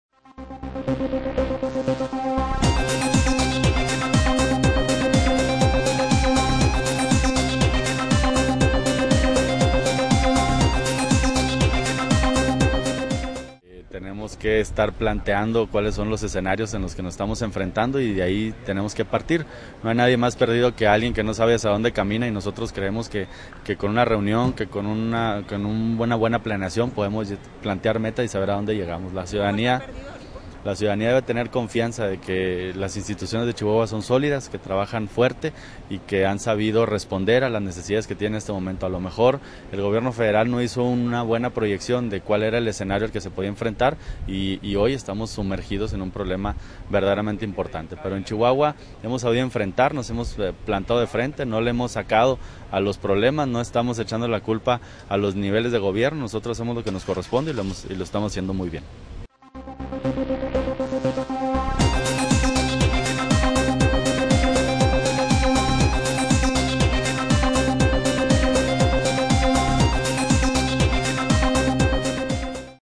Ciudadanía de debe de confiar
Los gobernadores de los estados y el propio César Duarte y la federación deben atender a una estrategia única para hacer frente a la inseguridad que aqueja a todo el país y que permita trabajar a favor de los mexicanos, dijo el diputado Jorge Ramírez, unos minutos antes de ingresar al recinto de Palacio de Gobierno.
Agregó que el trabajo ordenado, a través de un solo mando es el que nos puede dar la ventaja en el combate a la delincuencia, de lo contrario, es poco lo que se logrará avanzar, puntualizó.